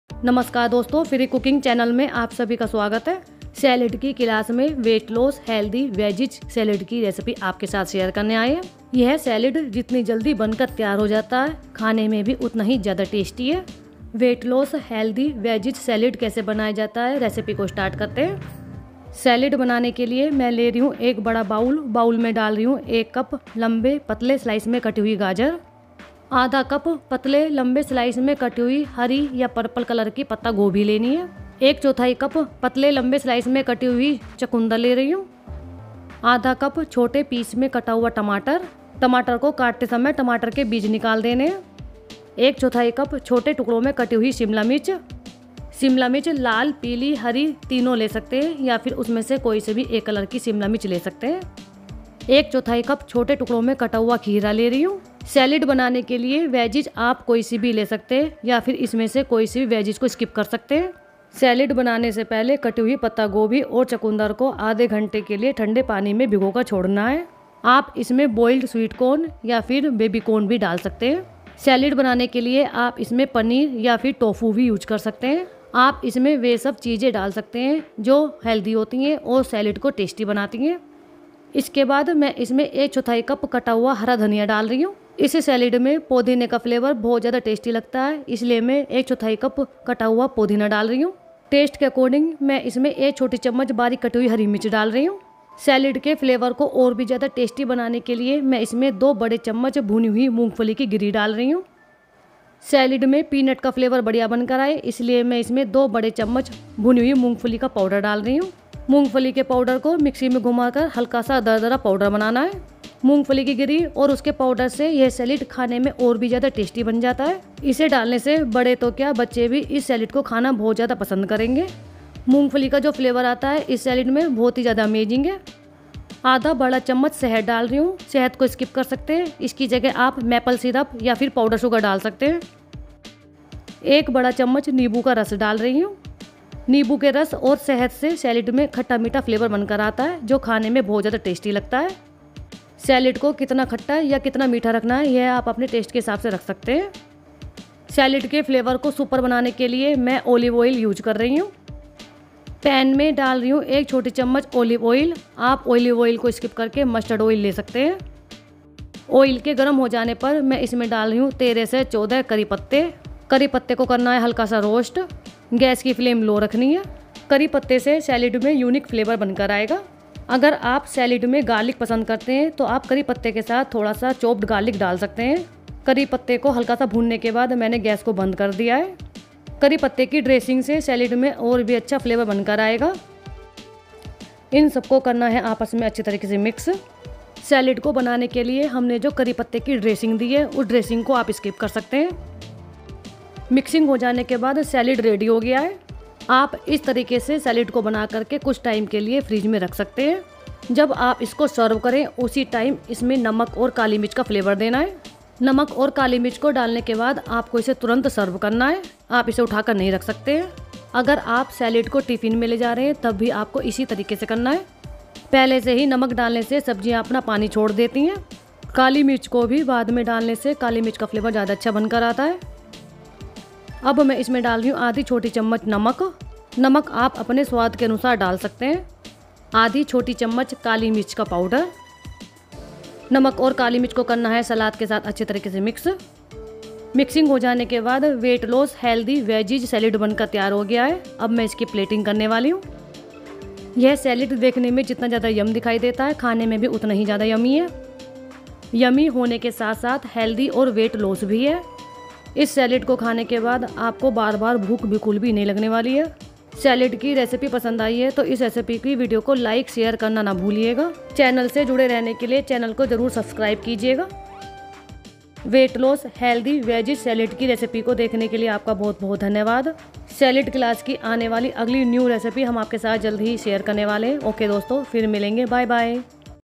weight loss healthy veggies salad इस सलाद को खाने के बाद आपका वेट जरूर कम होगा और आप बनोंगे हैल्दी
This salad will definitely reduce your weight.
Reduces weight and makes your body healthy.
You can prepare it very quickly.
it digests very quickly.
Available in a variety of healthy flavors.
इस सलाद से आपका वेट जरूर कम होगा।
वेट कम करता है और आपके शरीर को हेल्दी बनाता है।
इसको आप बहुत जल्दी बनाकर तैयार कर सकते हैं।
यह बहुत जल्दी हजम हो जाता है।
इसमें बहुत सारे अलग-अलग हैल्दी फ्लेवर दिए गए हैं
Ingredients -
Slice of Carrots - 1 cup
Slice of Cabbage - 1/2 cup
Slice of Beetroot - 1/4 cup
Chopped Tomato - 1/2 cup
Chopped Capsicum - 1/4 cup
Chopped cucumber - 1/4 cup
Chopped green coriander - 1/4 cup
Chopped mint leaves - 1/4 tsp
Roasted peanuts - 2 tbsp
Roasted peanuts powder - 2 tbsp
Powder sugar/ honey - 1/2 tbsp
olive/ mustard oil - 1 tsp